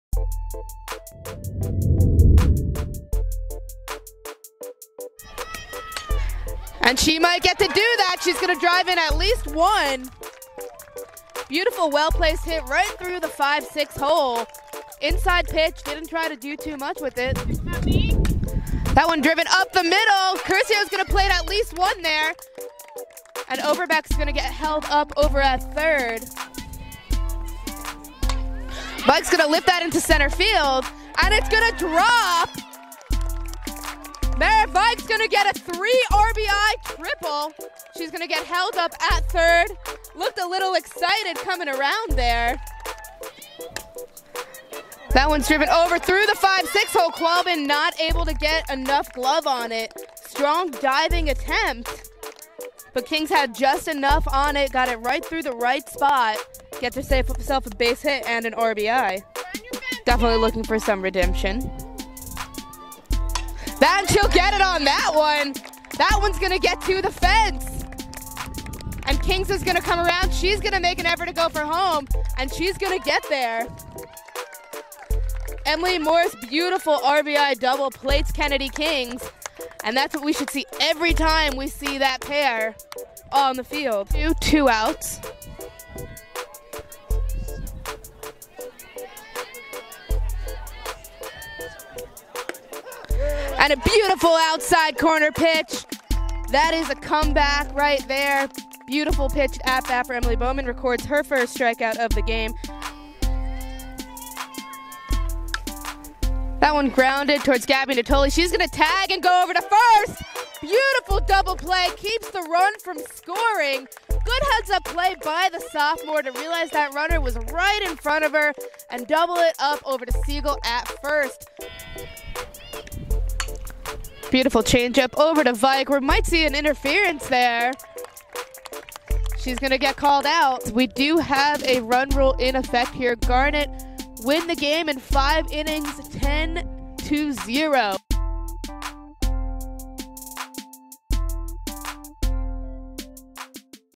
Swarthmore Softball Highlights vs. Muhlenberg (April 21, 2018)
Highlights from Swarthmore's 10-0 win over Muhlenberg on senior day.